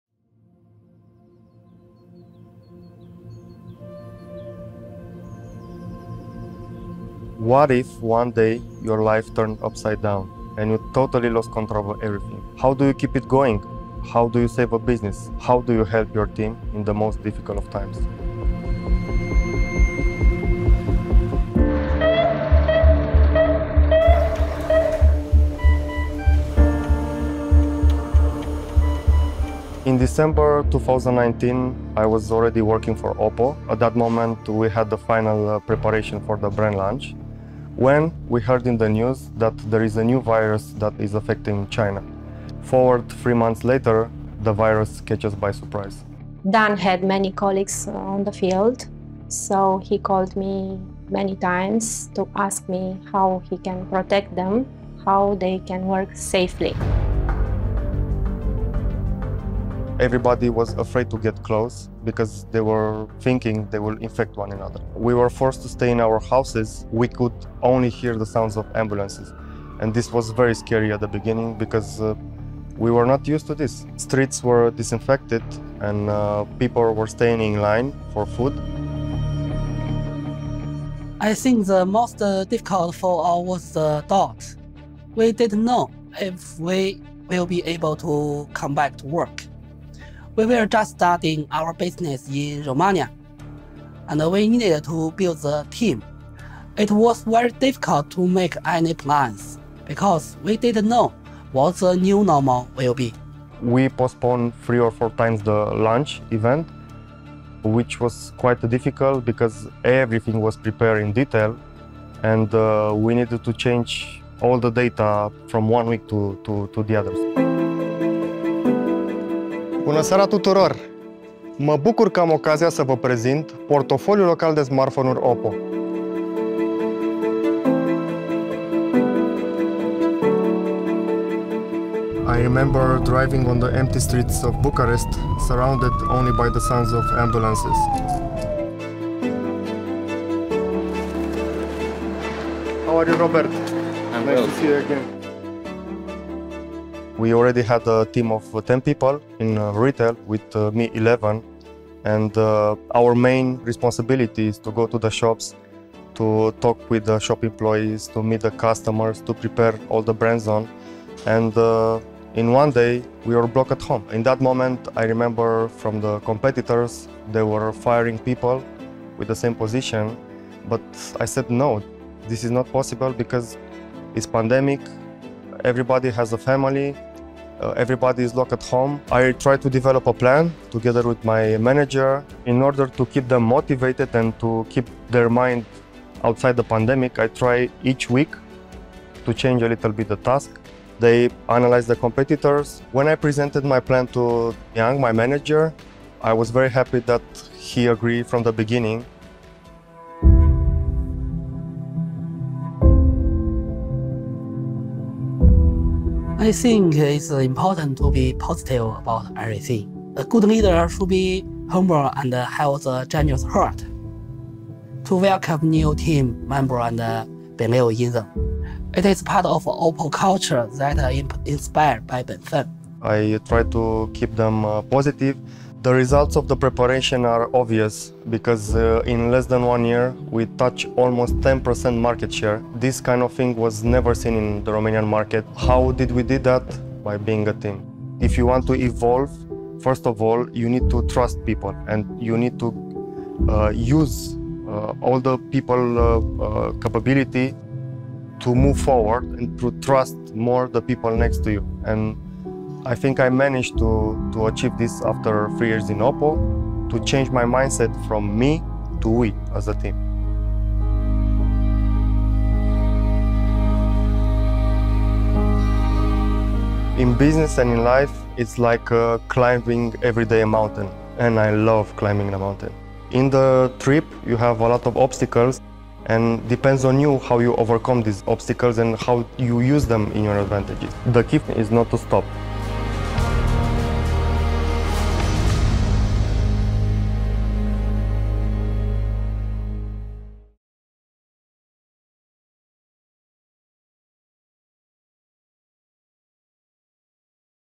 OPPO’s Inspiring People | Dan's Story -- مُلهمين أوبو | قصة دان
“لو فريقك حس أنك موجود معاهم، هتعرفوا تعدوا بسلاسة في أي موقف”
الوباء العالمي كان صعب على الكل. بالنسبة لرئيس قسم البيع بالتجزئة في رومانيا، كان عنده تحدي إزاي يخلي فريقه متحمس للشغل في وقت الوباء الصعب. قرر  يديهم فرصة يتكلموا ويعبروا عن نن نفسهم بحُرية عشان يحسوا بالأمان

في أوبو ، نعتقد أن العمل الجماعي دايما بيشجع على التحفيز. مع ميديرن زي دان، هنقدر نكمل في الإلهام